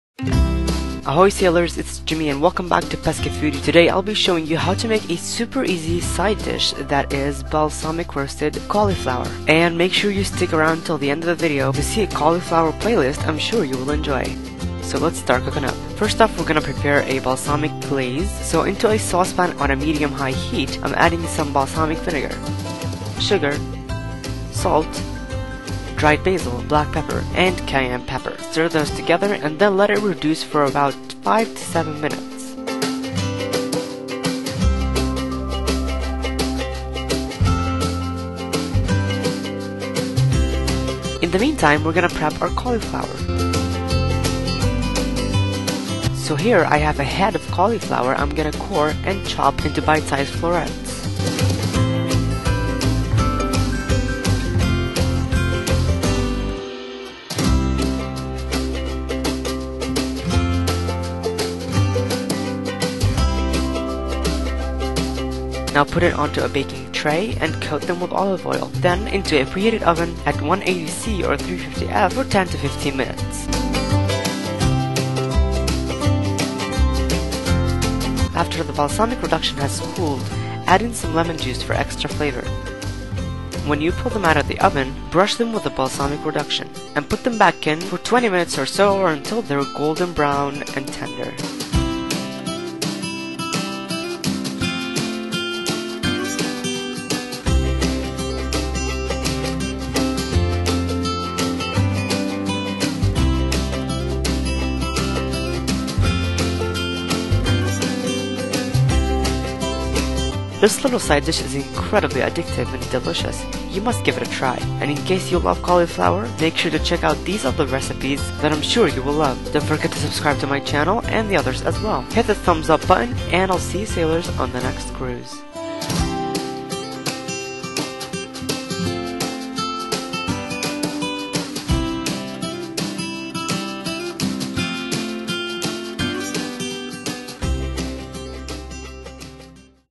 Balsamic Roasted Cauliflower | PescaFoodie ⚓ Episode 22
⚓ Ahoy sailors, and welcome to PescaFoodie!
On this episode of PescaFoodie we're making some balsamic roasted cauliflower!
They're super addictive and go well in salads or as a side dish even!!

Recipe: [serves 2 as a side]
a medium sized head of cauliflower
¾ balsamic vinegar
1 tsp of dried basil
½ tsp of cayenne pepper

Baking temperature: 180C / 350F
Baking time: 10-15 minutes & 20 minutes

⚓ Tastemade Network Partner